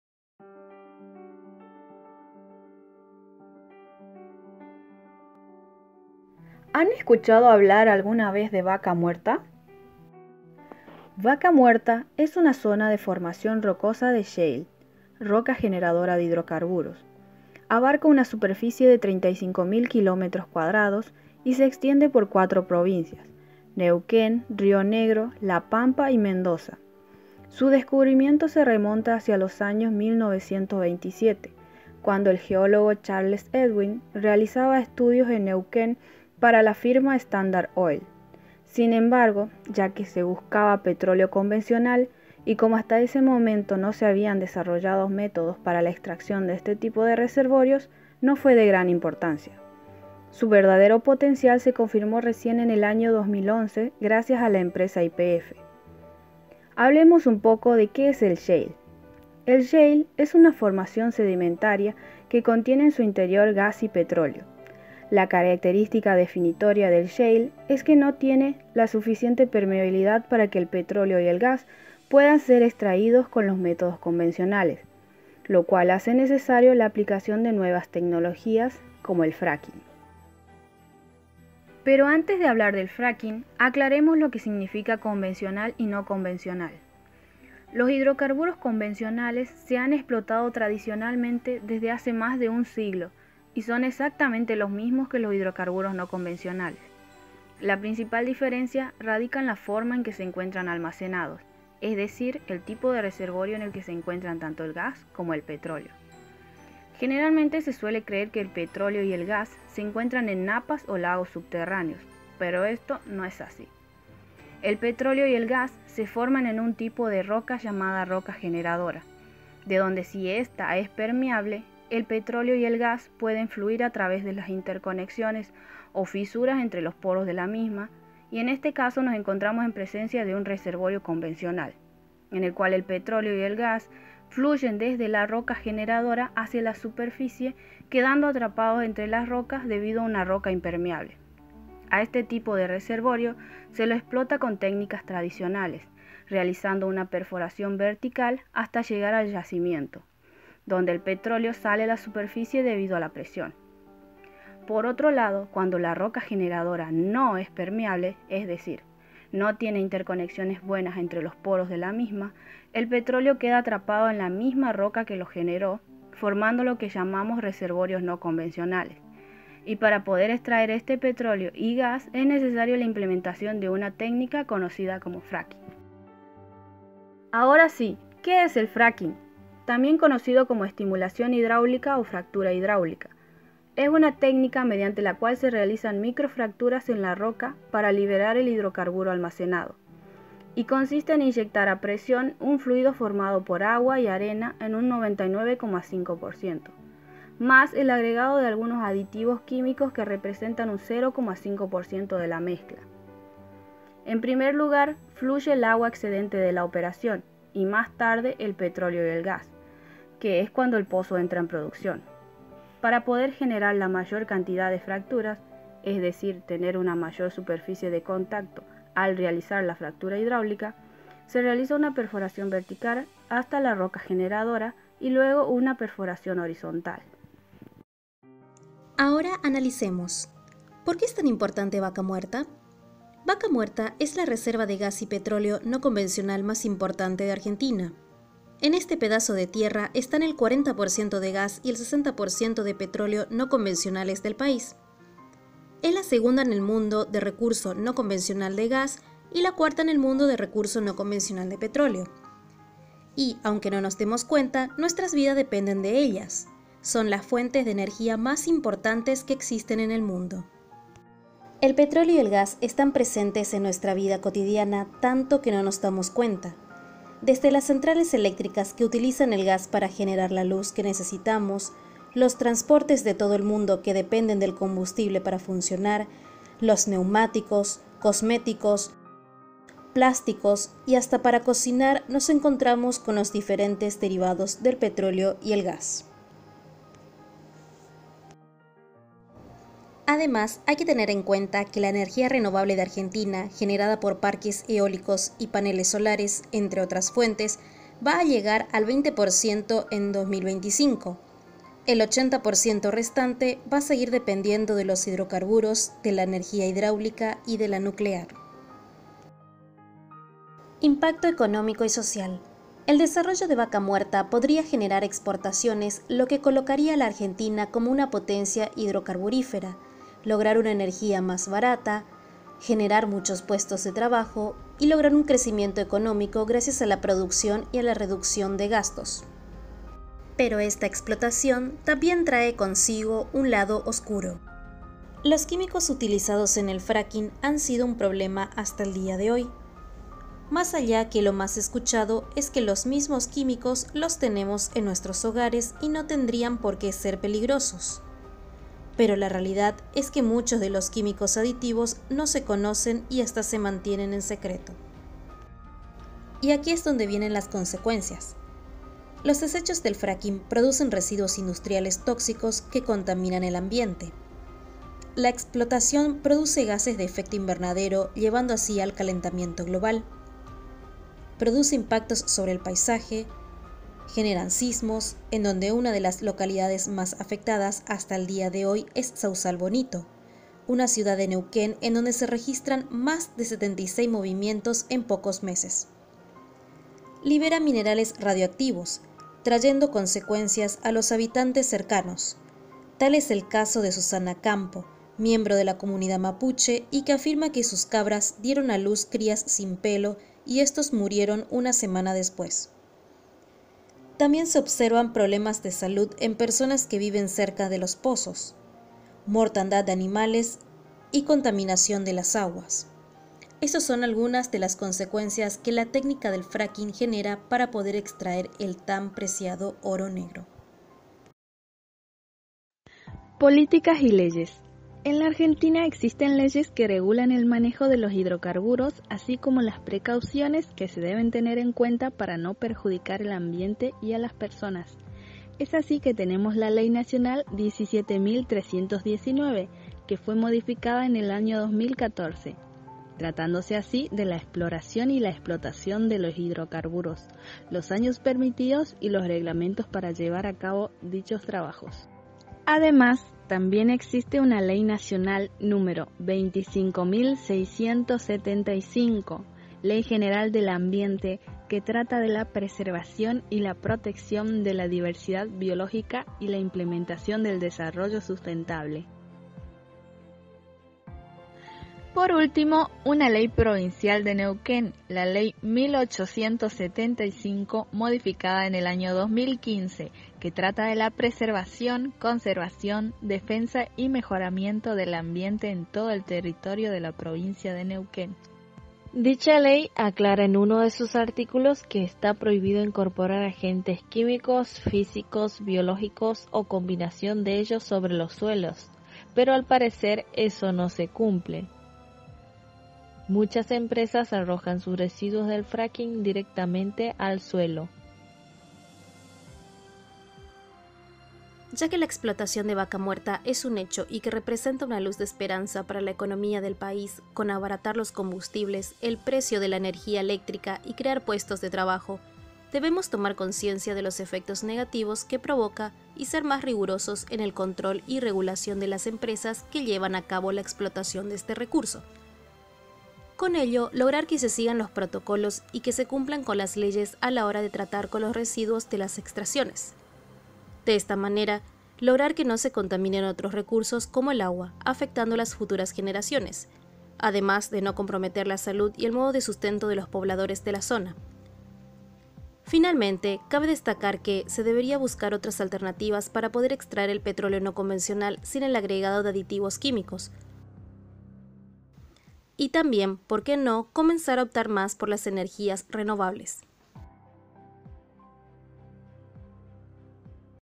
EN BUSCA DEL ORO NEGRO
Trabajo realizado por los alumnos de CTS_2019
Vaca Muerta
Tema 6: Desarrollo Sostenible. Causas y Problemas